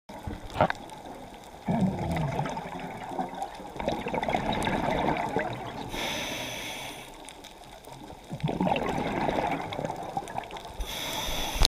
Juvenile Rock Mover Wrasse
Our first time seeing one of these cute, little fish.